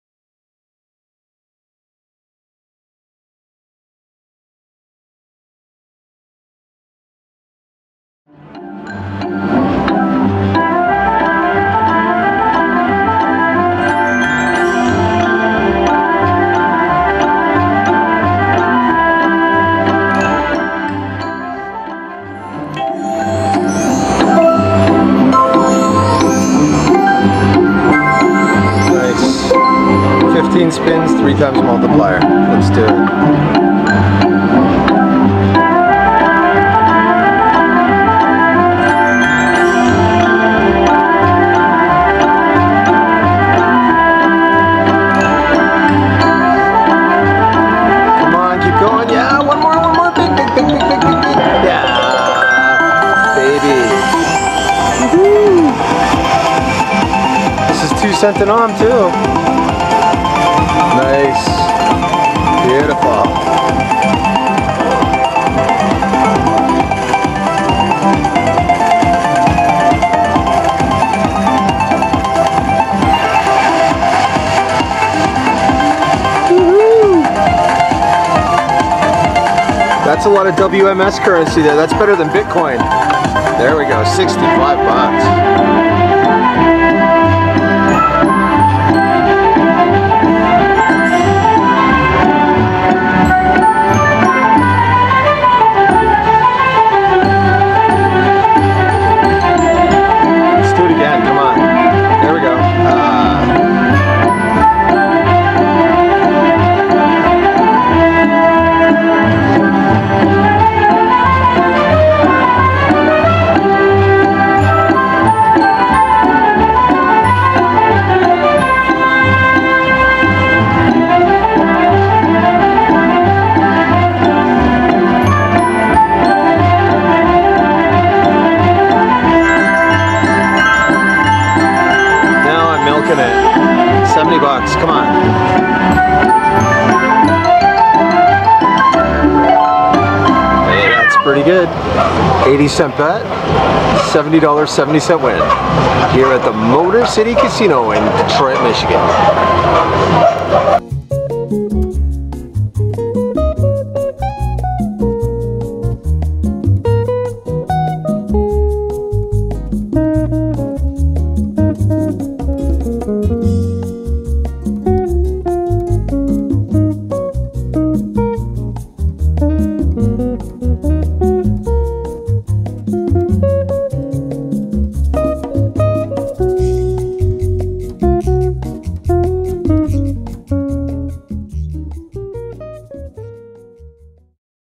WMS - OMG! Kittens! Nice win! Meow!
Set your slots to CUTE OVERLOAD, because we are going in!  It's OMG! Kittens!  Played at the Motor City Casino in Detroit, MI.